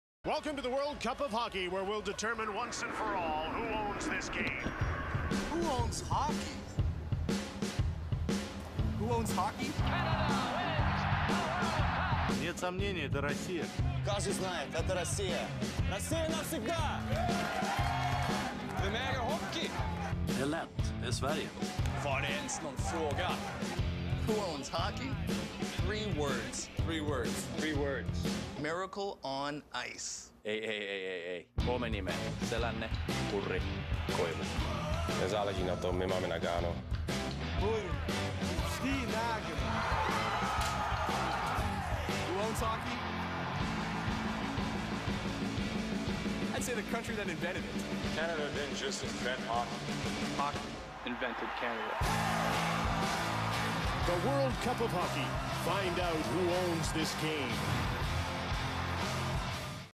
World Cup of Hockey Commercial 2016 M Models and Talent Agency
M Models and Talent Agency

Talents used: Karyna P., Sebastian D., Sebastian N. and Phoebe G.